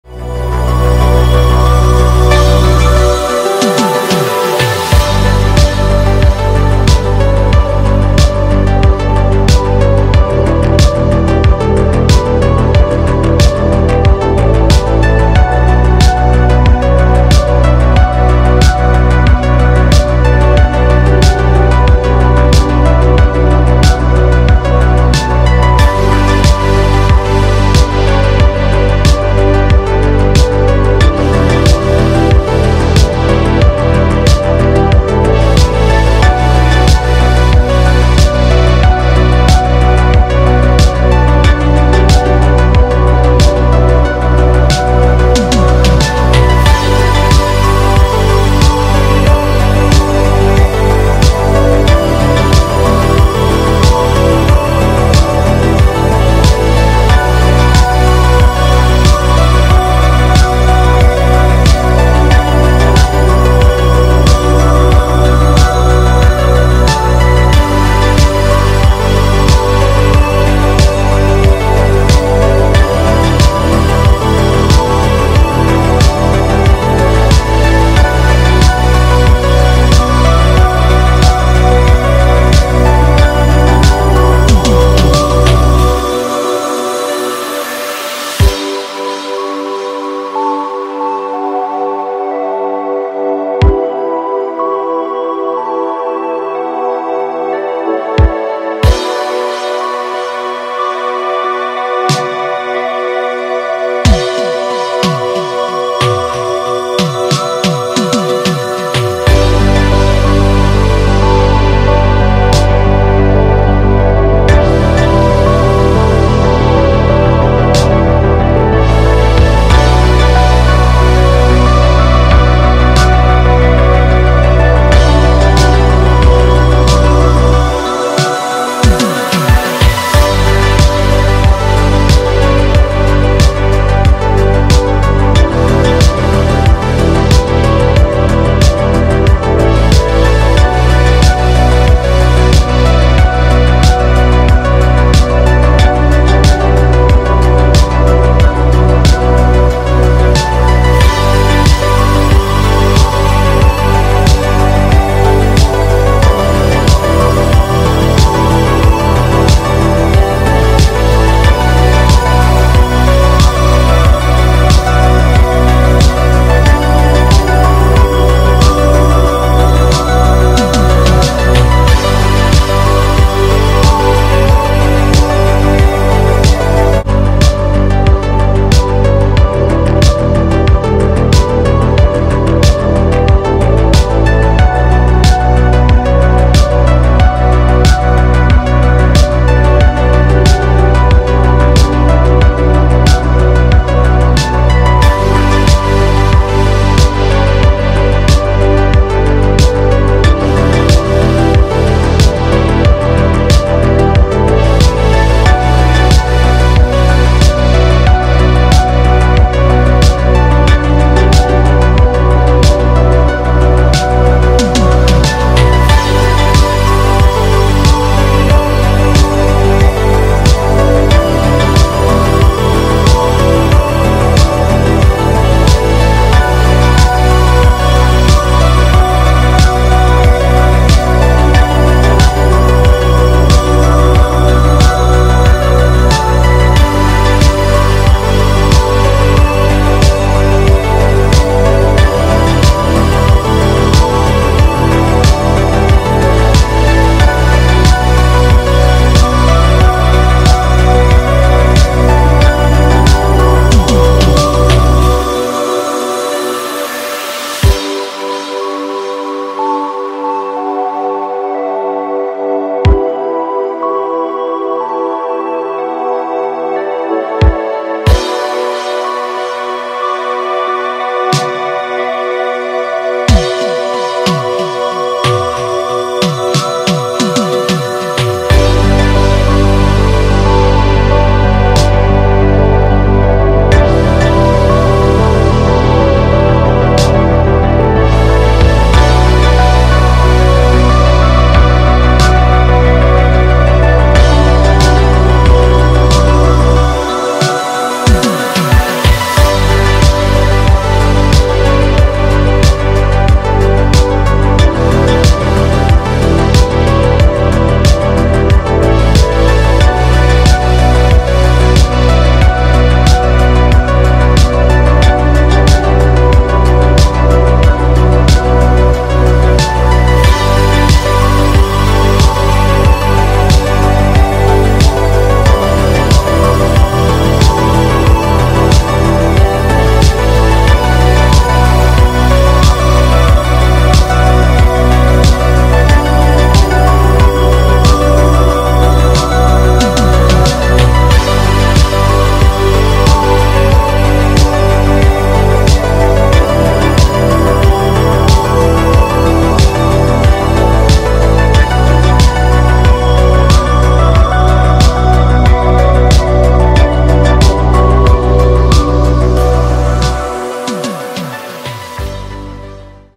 Mexico in photo. Video 4K.
Mysterious Mexico.  Beautiful nature.  There are deserts and jungles.
Загадочная Мексика. Красивая природа. Есть и пустыни и джунгли.